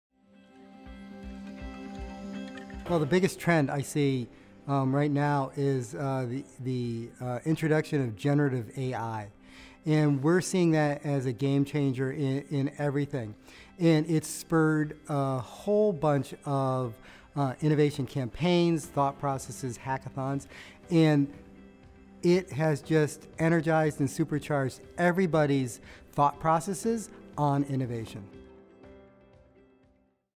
The Future Of Innovation Management: The AI Revolution
If you've been keeping a close eye on the latest trends, you probably can't miss the rise of generative AI. It's transforming the landscape of technology and innovation management, creating a whirlwind of excitement and creativity.

Bob Goldman, Principal Innovation Strategist at CVS Health, explores how generative AI is revolutionizing everything and sparking a further development of innovation management!

Read the full article "The Future of Innovation Management: Trends and Predictions"